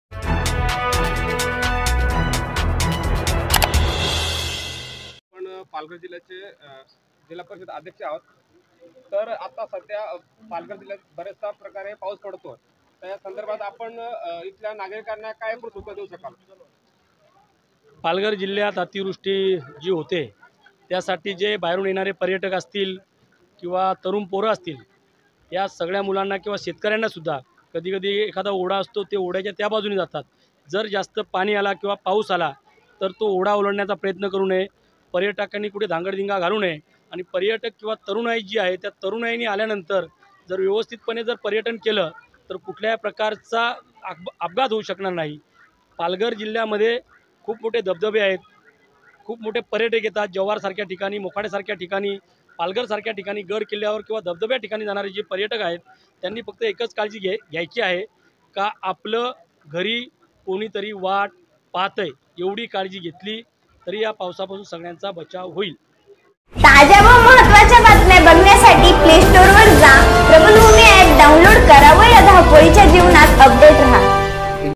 पालघरमध्ये प्रचंड पाऊस; नागरिकांना सतर्क राहण्यास आव्हान
पालघरमध्ये प्रचंड पाऊस; नागरिकांना सतर्क राहण्यास आव्हान; पालघर जिल्हा परिषद अध्यक्ष प्रकाश निकम यांचे आवाहन